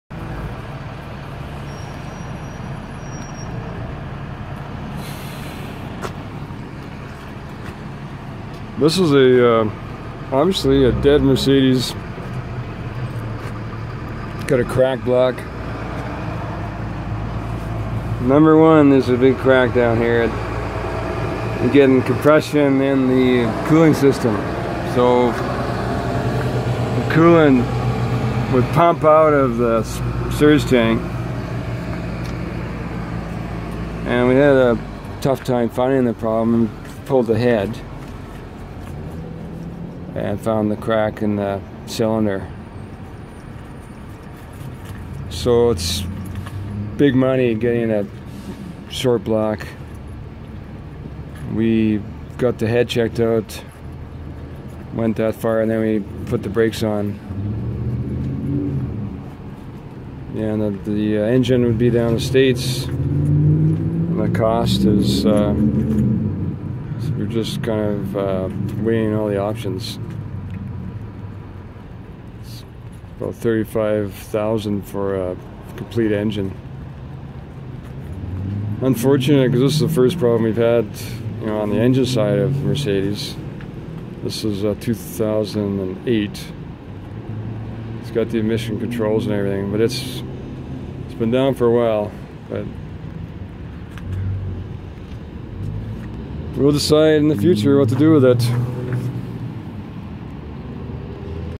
Mercedes Diesel Engine Cracked Block
2008 Mercedes MBE 900 in a Thomas HDX pusher bus. Unfortunately the number one cylinder has a crack. Originally coolant was pressuring up and blowing out the coolant surge tank.
We ended up pulling the cylinder head and getting it checked out. At the same time thinking it could be a head gasket but finally saw the crack which threw a wrench into our plans.

A short block was hard to come by or not available. The long block from the U.S. was priced at $35,000 plus shipping plus tax etc. There were lots of pre-emission engine blocks at  the wrecking yards we contacted but nothing that we could use since 2007 engines and newer are emission controlled with a totally different block. So... here it sits in the middle of our yard waiting for a solution.